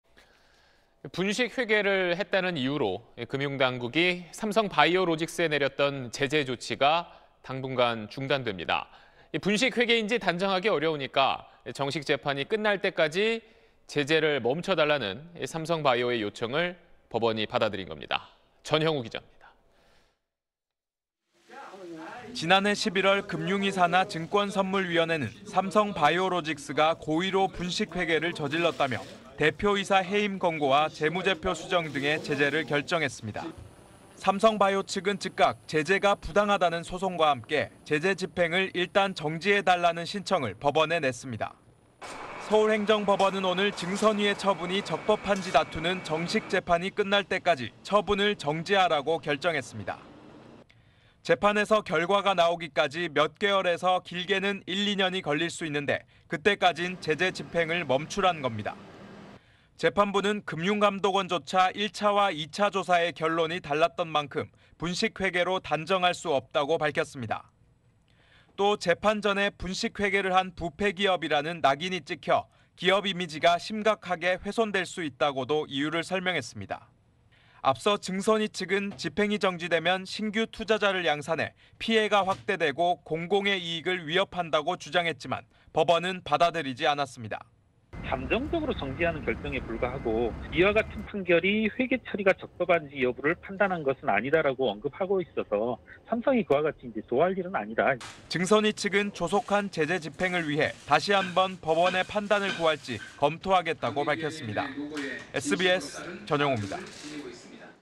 '분식회계' 삼성바이오 제재 중단…"위법 단정 어렵다" / SBS
분식회계를 했다는 이유로 금융당국이 삼성바이로직스에 내렸던 제재 조치가 당분간 중단됩니다.

분식회계인지 단정하기 어려우니까 정식 재판이 끝날 때까지 제재를 멈춰달라는 삼성바이오의 요청을 법원이 받아들인 겁니다.